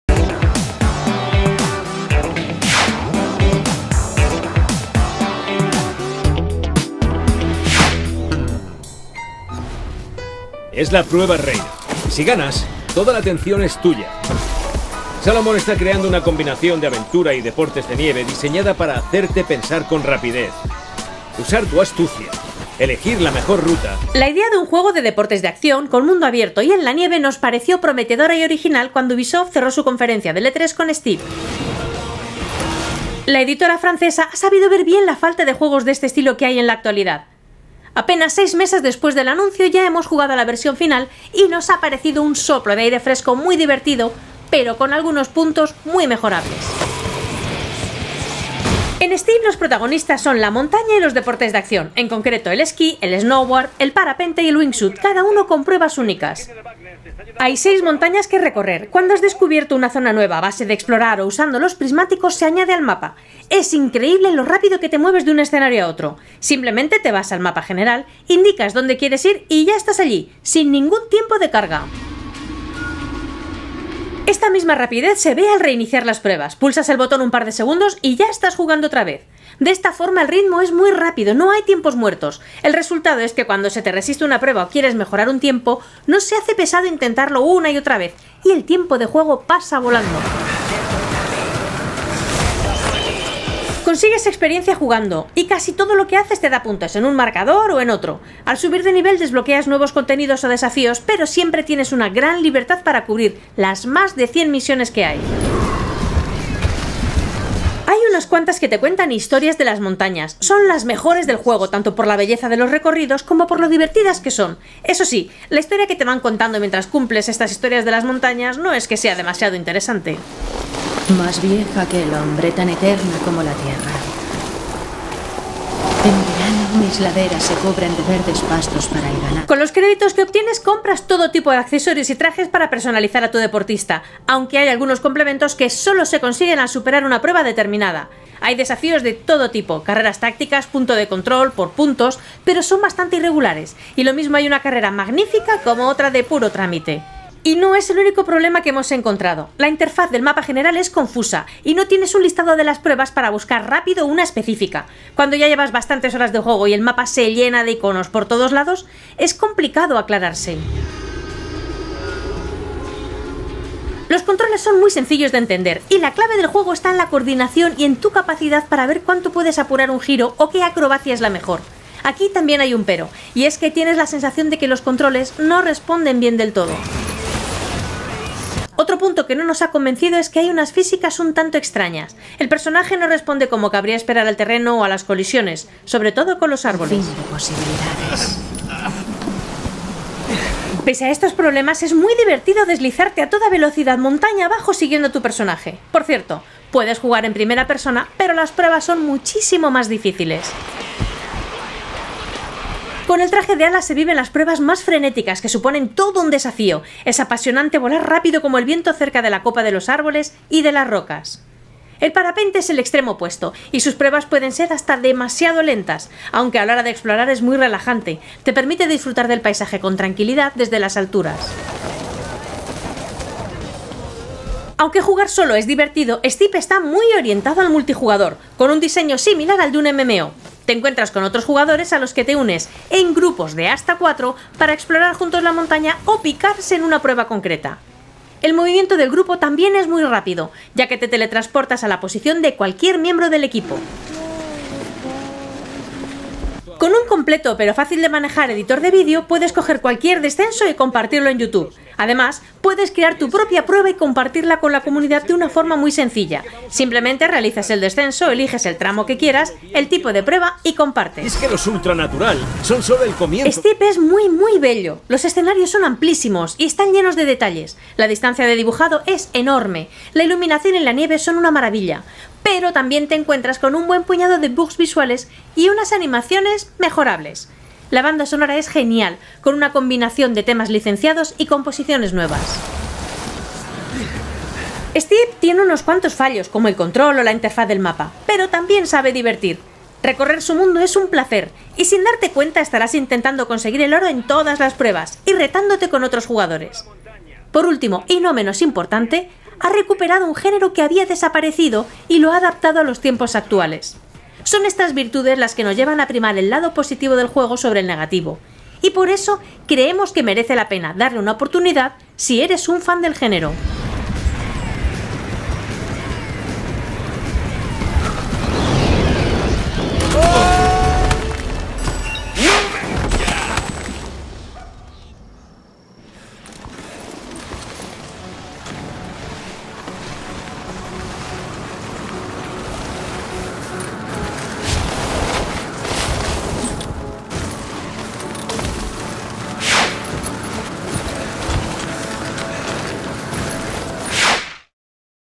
Vídeo Análisis de Steep
Ubisoft revive con acierto los juegos de deportes de acción con un divertido título de mundo abierto repleto de contenidos.